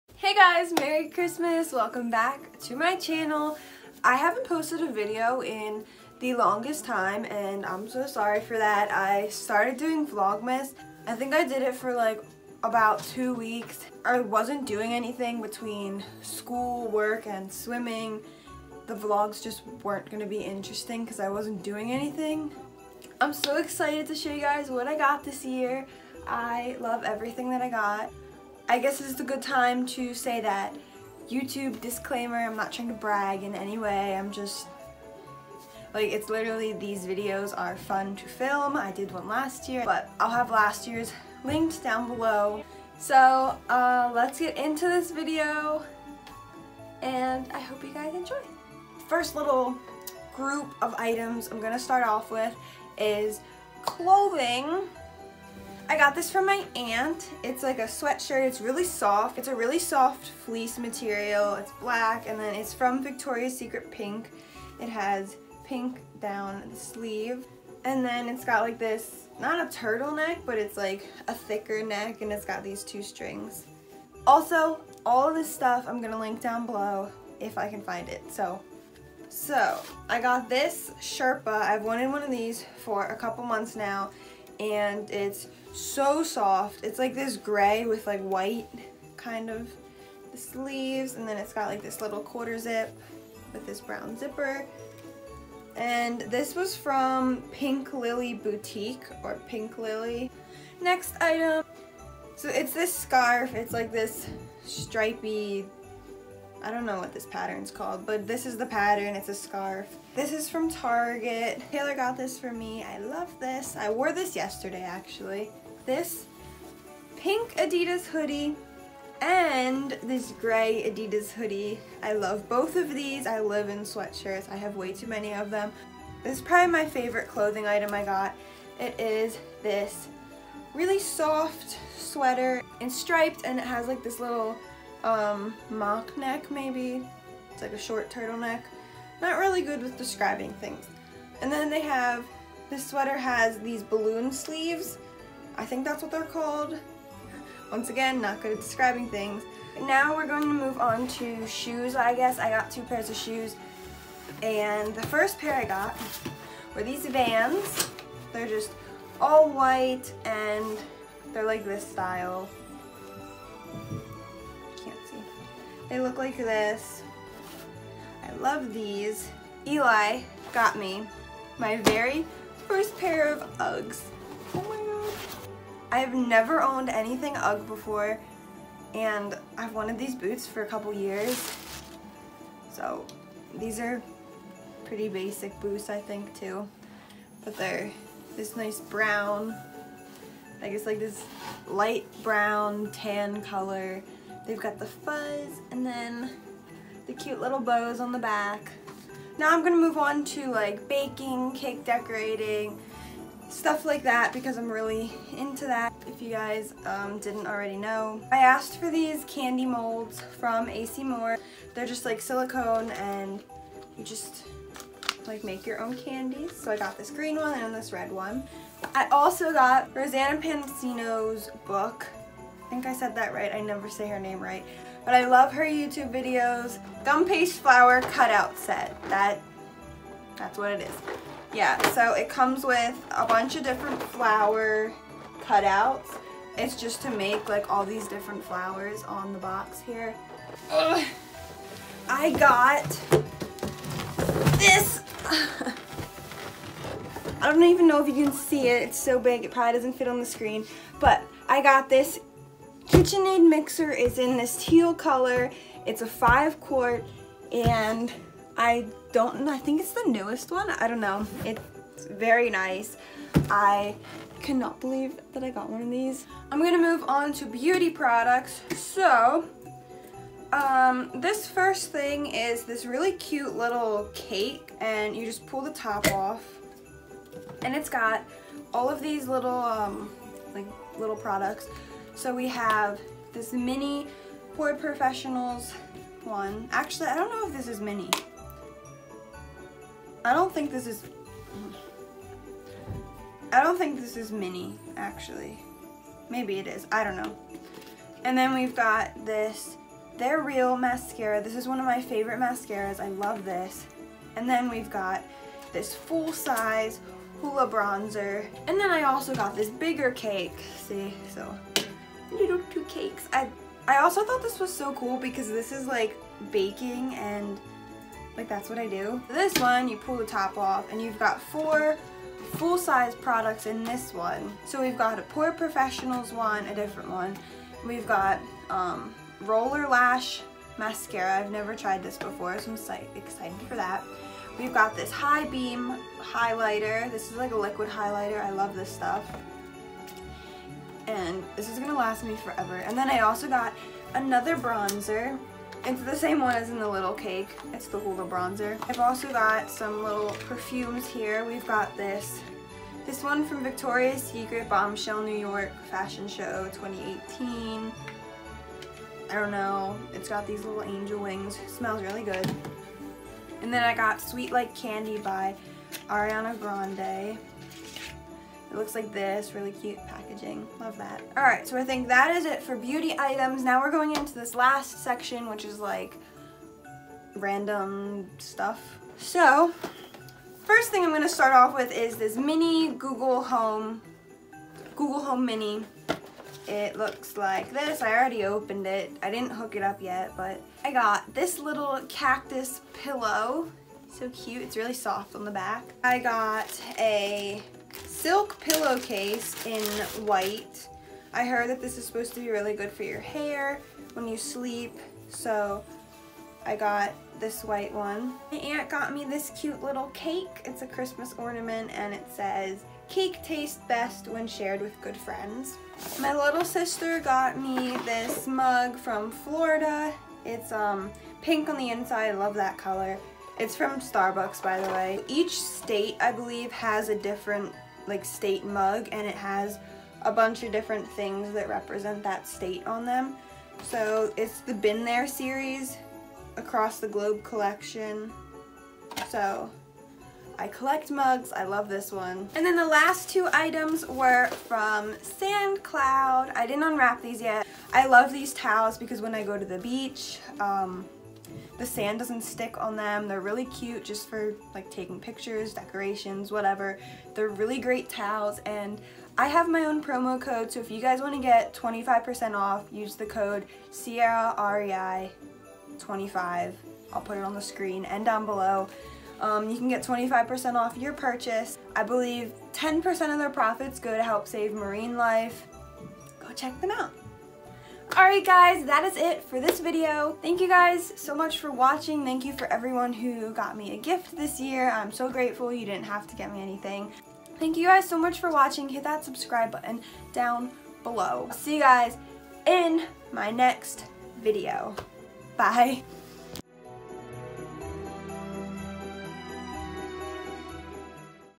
WHAT I GOT FOR CHRISTMAS 2018 // cieramist
Hey everyone! This is one of my favorite videos to film! I hope everyone had a great holiday!

Most of the items shown in this video are linked below!

candy molds - (similar)
rosanna pansino book
kitchen aid mixer
pink benefit cosmetic cake
ariana grande perfume
google home mini
white silk pillow case
starbucks mug